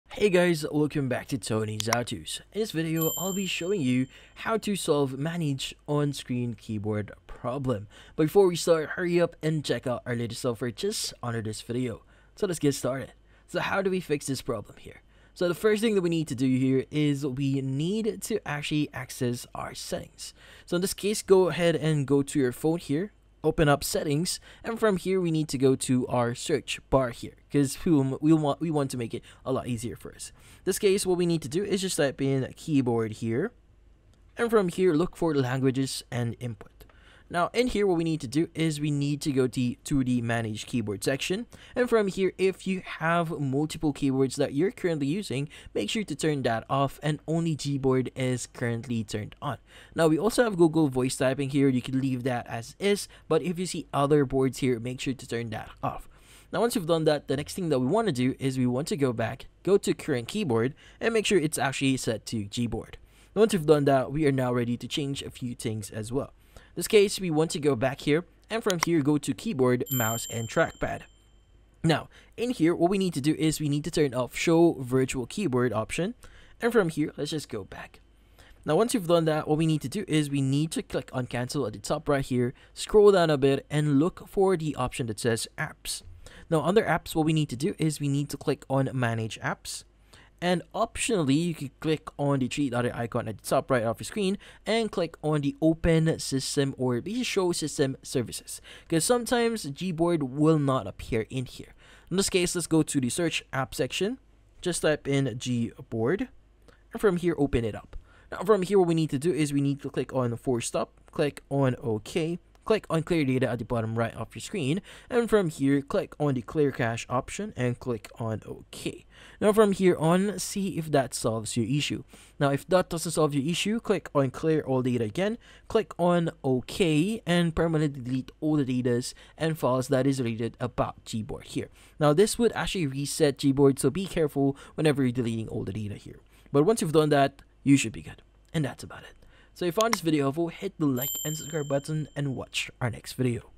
How to Solve 'Manage On Screen Keyboard' Problem | Manage On Screen Keyboard Problem Fix (2024)
In this video, I show you How to Solve 'Manage On Screen Keyboard' Problem | Manage On Screen Keyboard Problem Fix in 2024. The quick and easy tutorial How to Fix Manage On Screen Keyboard Problem On Android. Do you want to know step by step On Screen Keyboard Problem Solved in 2024? Yes? Great!